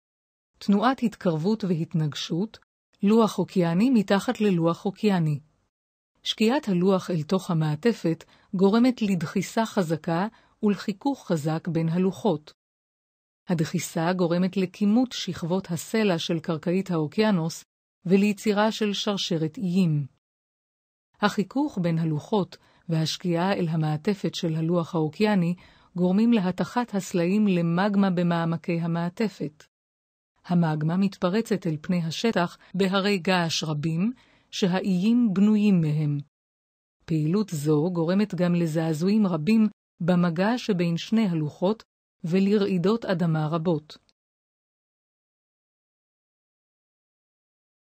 התנגשות לוחות באוקיינוס
התנגשות לוחות אוקייניים (באוקיינוסים) בדומה ליבשת, פרטים להלן.
התנגשות לוחות טקטונים - לוחות יבשה גורמת לדחיסת הקרקע ויצירת שרשרת הרים געשיים" .
שלבים בהיווצרות גבול פתיחה:
זרמי ערבול במגמה שנמצאת במעטפת העליונה (אסתנוספירה) פורצים במערכות סדקים בקרום הימי של כדה"א.
המגמה פוגשת את המים הקרים יותר של האוקיינוס.
המגמה מתקררת ומתגבשת לסלעים. כך נוצר קרום אוקייאני חדש.
קרום זה דוחף את היבשות לצדדים והן הולכות ומתרחקות זו מזו.

תנועת התכנסות והתנגשות- הסבר שיש כמה סוגים...
לוח אוקייני מתחת ללוח יבשתי.
דחיסה של לוח ימי מתחת ללוח יבשתי גורמת לחיכוך בין הלוחות
חיכוך זה גורם לחימום רב של תחתית הקרום.
קיים קימוט של שכבות סלע בעיקר בלוח היבשתי.
נוצרת יחידות נוף בשם שרשרת הרים.
החיכוך והנחיתה לאזורי חום ולחץ רבים גורמת להתכה בעיקר של הלוח הנוחת.
התכה זו לעליית מגמה בצינורות וסדקים כלפי מעלה.זוהי התחלתה של פעילות געשית.
המגמה פורצת והופכת ללבה בהרי געש. בנוסף התפרצויות אלו גורמות לרעידות אדמה.
לוח ימי מתחת ללוח ימי
1. ההתקרבות והתנגשות גורמות לדחיסת לוח מתחת ללוח אחר.
2. שקיעת לוח אחד מתחת לשני גורמת לחיכוך
3. בגלל הדחיסה נוצר קימוט בשכבות הסלע שמתחת לפני הים.
4. הקימוט הרב יוצר שרשרות איים מעל פני האוקיינוס/ים.
5. החיכוך גורם ליצירת חום שמתיך את הסלעים במעמקי המעטפת.
6. מגמה זו מתפרצת לפני השטח והופכת ללבה על פני השטח של האיים.
7. ההתנגשות והחיכוך גורמות לרעידות אדמה.
התנגשות בין יבשות
1. קרום ימי שנמצא בשולי יבשת נדחק ונעלם מתחת לקרום יבשתי.
2. קרום ימי זה נעלם וכך גם הים/אוקיינוס שהפריד בין שתי היבשות.
3. לאחר שהקרום הימי נעלם נפגשים שני הקרומים היבשתיים.
4. אחד מהקרום שוקע ונדחס מתחת לקרום הקל יותר. דחיסה זו גורמת לפעילות מגמתית (געשית), רעידות אדמה וקימוט רב.
5. קימוט זה יוצר שרשראות הרים גבוהות ביותר (הימלאיה, רוקיס, אנדים).

ים סוף ובקע ים המלח
1) רכס מרכז אוקייני (נקרא גם "גבול אזור פתיחה") עולה במרכז ים סוף.
2)  עלייה זו מפרידה את הלוח האפריקני מהלוח הערבי.
3) פתיחה זו גורמת גם לתנועה אופקית לאורך השבר ממפרץ אילת ועד החרמון.
4) שני הלוחות נעים צפונה אך הלוח הערבי זז מהר יותר צפונה בקצב של 1.8 ס"מ בשנה.
5)  תנועה זו גורמת לרעידות האדמה הגדולות בישראל.
6) בעוד כמה מיליוני שנים ים סוף יפתח ויתרחב. הים התיכון יחובר לאוקיינוס ההודי ואנו נתרחק מאפריקה.
תנועת היבשות ע"פ וגנר
1)לפני 225 מליון שנים הייתה יבשת אחת בשם פנגיאה.
2) פנגיאה התפצלה לשתי יבשות- לאורסיה (צפונית כי אסיה כרגע בצפון הכדור)וגונדוונה (בדרום הכדור).
3) במשך מיליוני השנים היבשות להתפצל עד שהגיעו למצב הנוכחי כיום.
4) מצב זה ממשיך להשתנות.